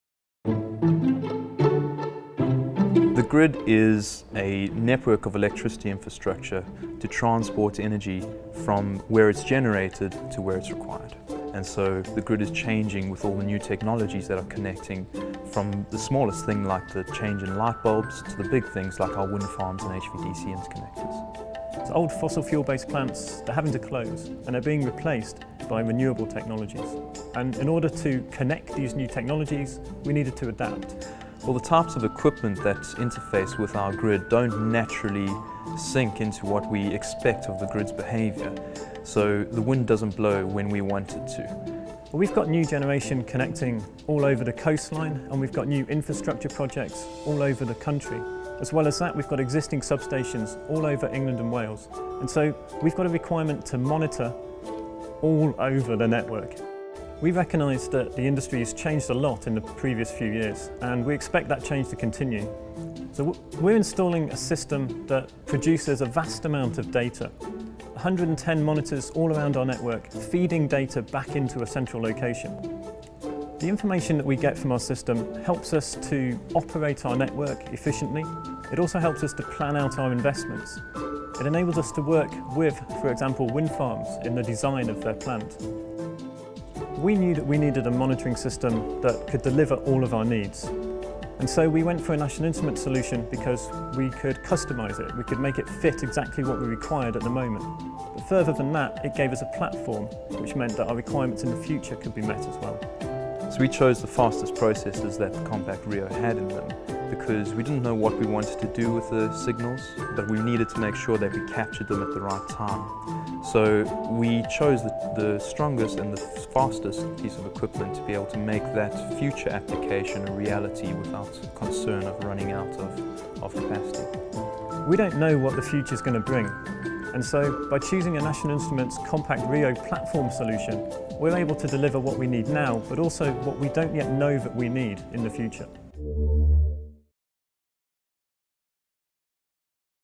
迎接新世代電網
英國國家電網公司是一家輸電系統營運商，在英國服務的客戶將近兩千萬人。目前，該公司採用 NI 平台，佈署了一套進階且具備升級功能的電網量測系統，可提供更理想的營運資料以維護英國電網的健全狀態。